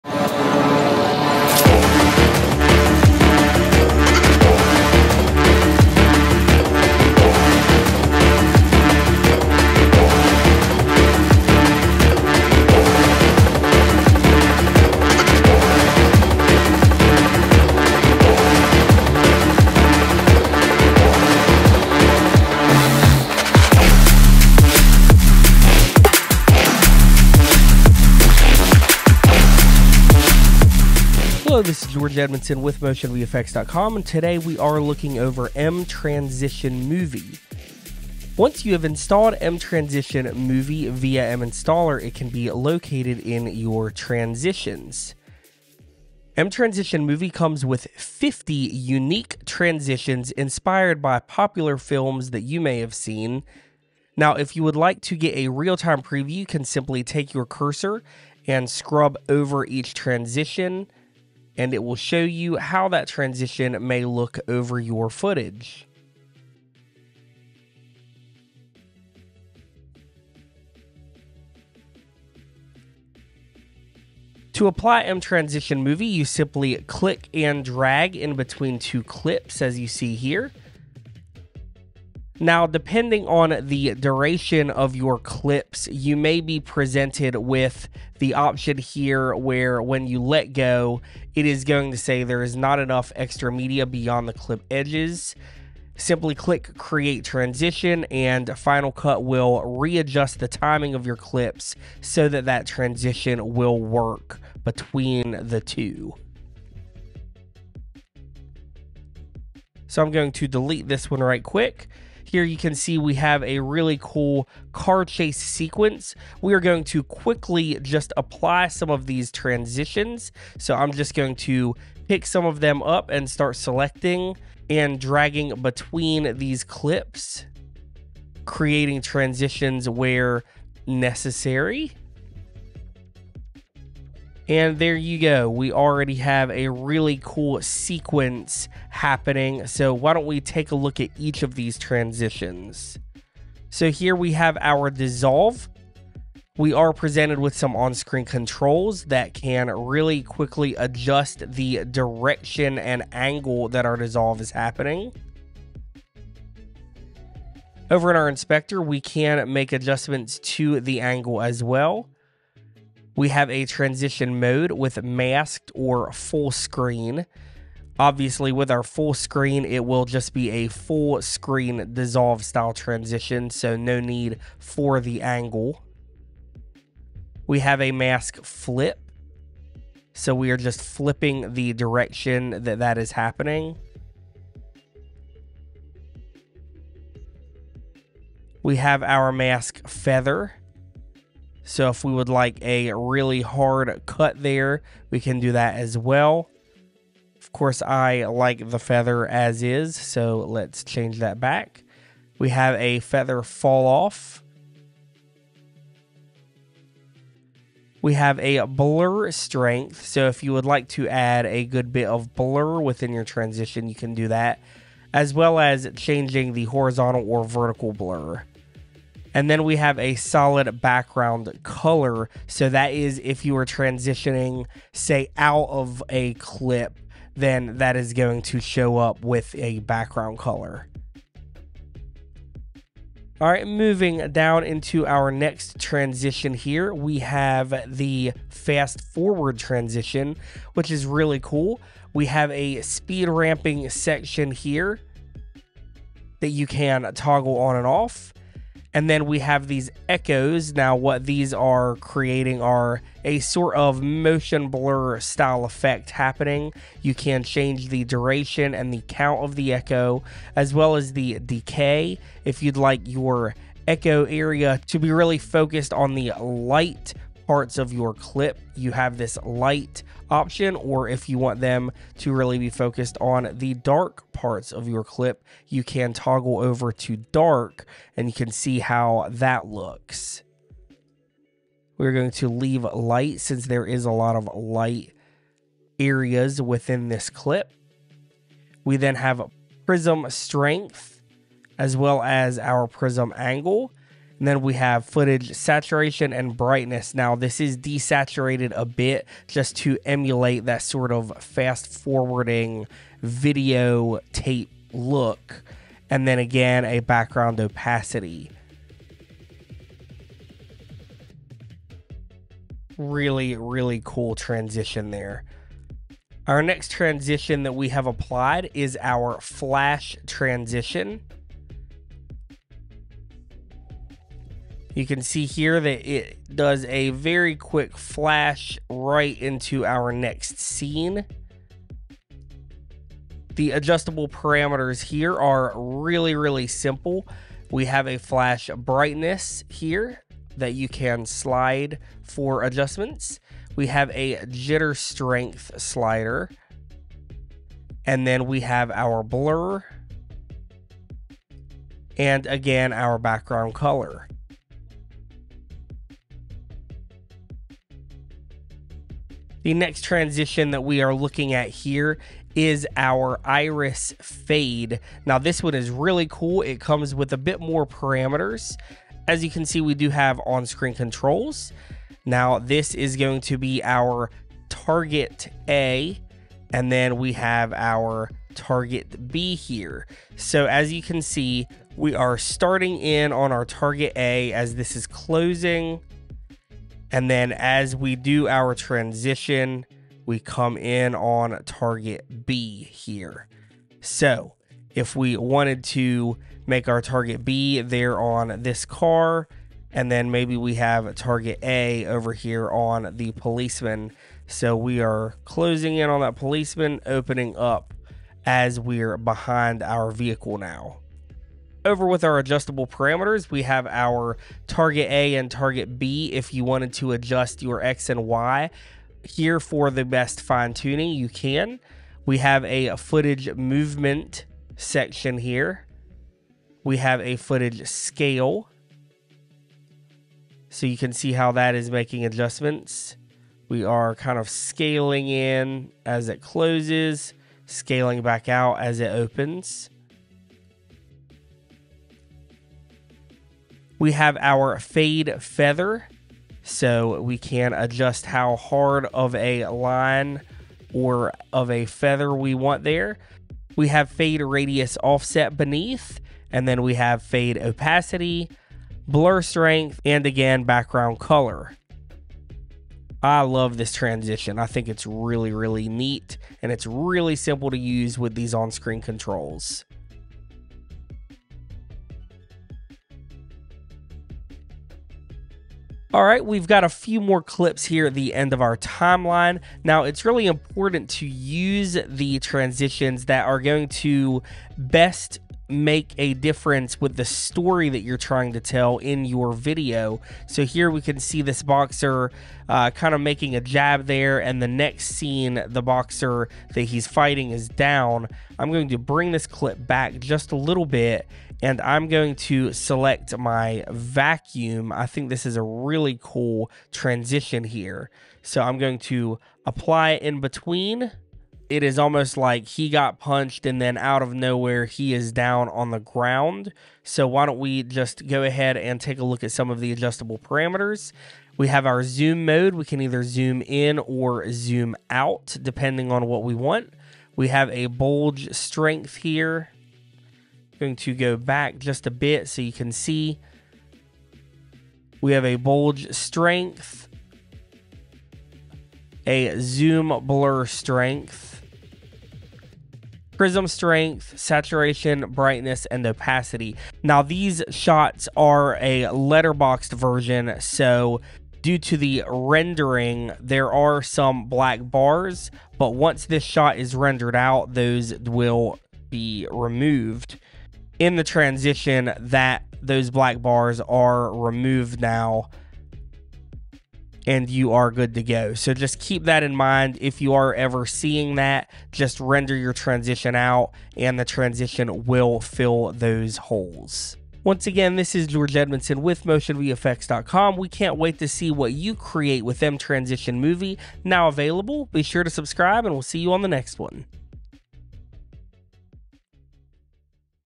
mTransition Movie Tutorial — Move between shots like a pro — MotionVFX
Check out the tutorial by George Edmondson as he shows you around the mTransition Movie — a pack of 50 classic cinema-inspired transitions that can help you enrich your narrative and get that straight-out-of-movie-screen look. Learn where to find the transitions after installation; let us walk you through some of them and get tips and tricks on customizing them to tell your story.

0:00 - Intro
0:32 - Location and pack’s overlook
1:14 - Applying transitions on your timeline
2:18 - Dissolve transition
3:50 - Fast Forward transition
5:15 - Flash transition
6:00 - Iris Fade transition
7:55 - Vacuum Transition